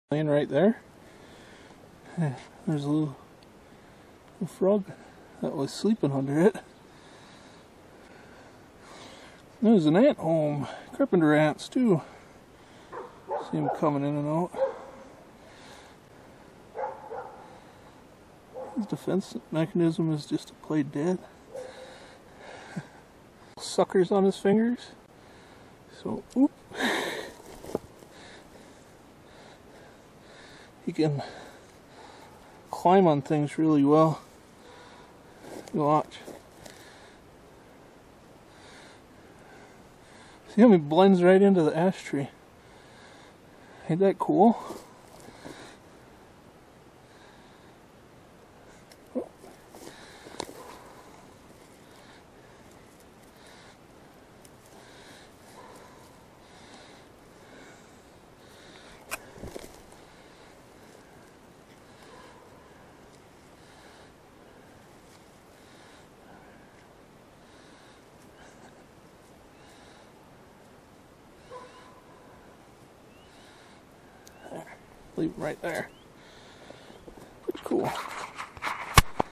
Gray treefrog Hyla versicolor "Color changing frog that climbs trees"
Just a quick video of a frog I found while working in the back yard.